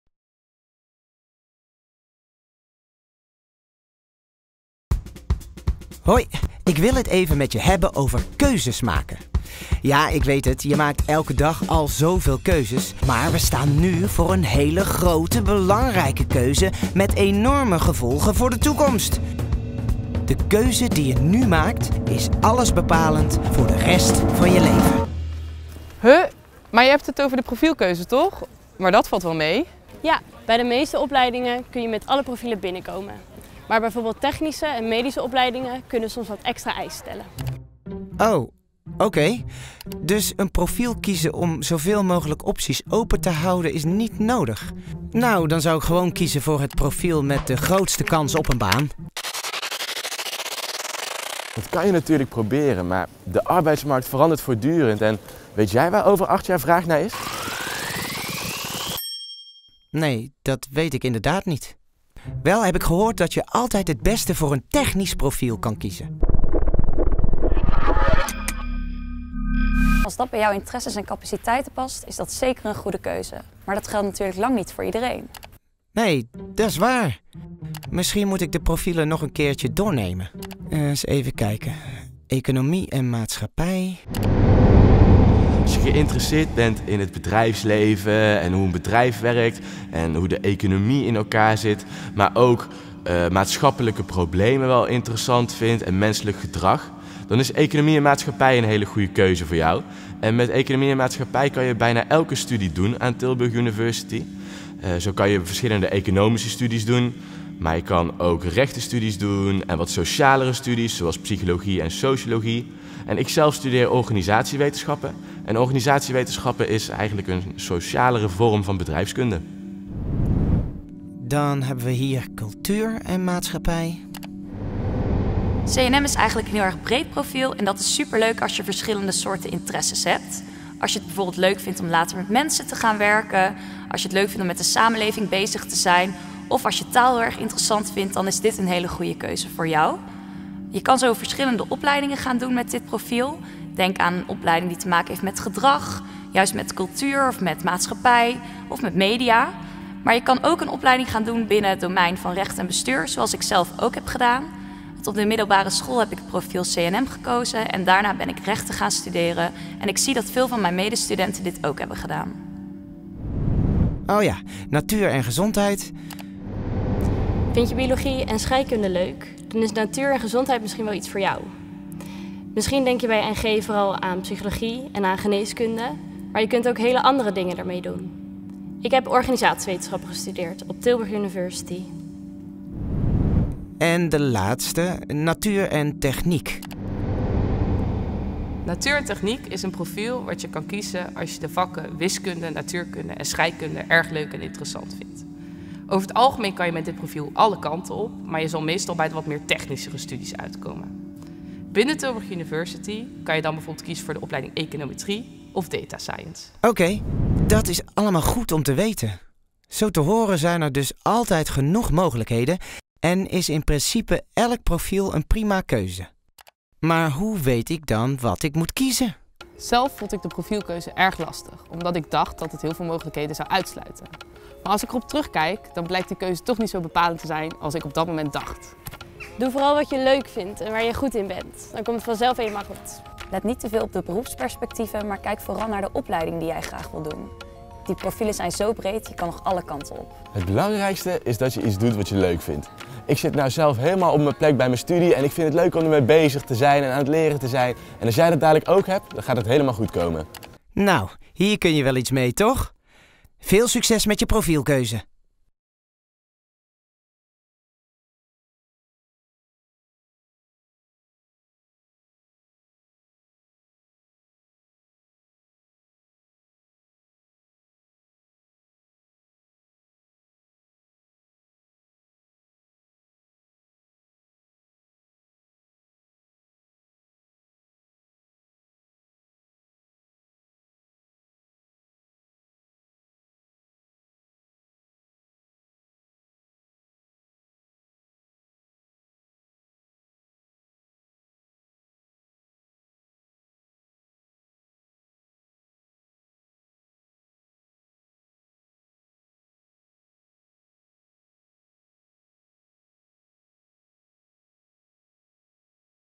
Profielkeuze vwo – Tilburg University
Waar let je op bij het kiezen van een profiel? Hoe belangrijk is je vervolgopleiding bij die keuze? En wat zijn de mogelijkheden qua vervolgopleidingen? Studenten van Tilburg University vertellen over hun ervaringen met de profielkeuze. Zij geven handige tips en laten ook zien dat profielkeuze en opleidingskeuze niet altijd overeen hoeven te komen. Elk profiel komt aan bod en we laten zien welke opleidingen daarbij aansluiten.